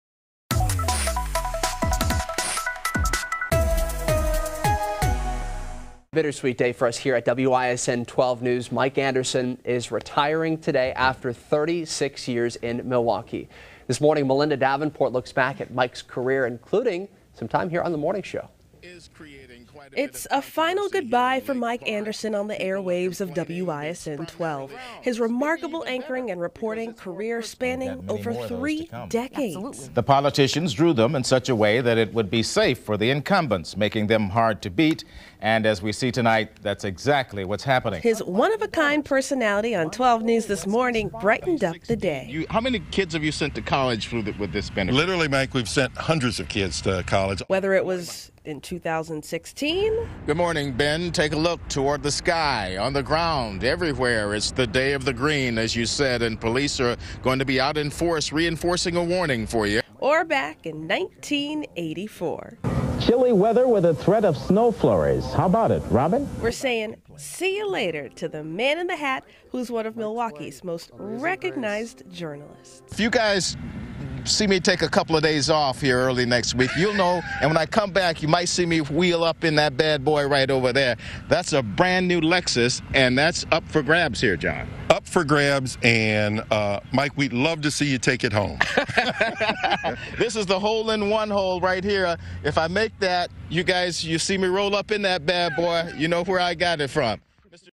Hats off! Mike Anderson retires Friday after 36 years
Join the "12 News This Morning" team in wishing the best to Mike Anderson as he retires from WISN 12 after 36 years. We'll miss you, Mike!